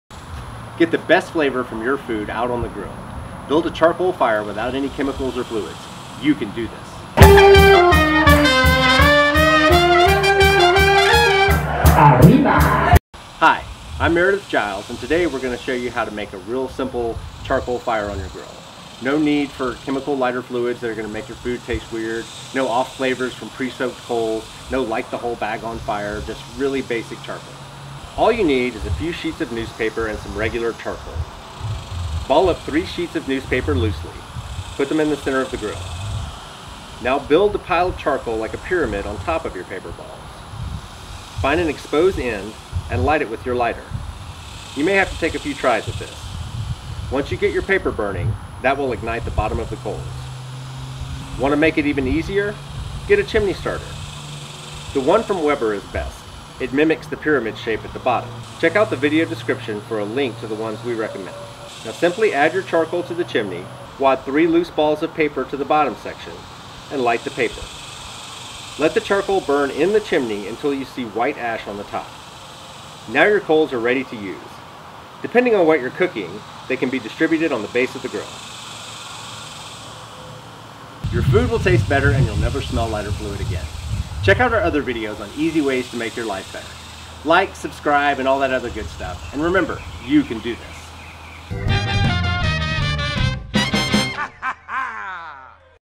How to Start a Charcoal Fire
How to start a charcoal fire without lighter fluid.
Skip the lighter fluid taste and let your meat taste like delicious meat.
Here is the chimney starter we recommend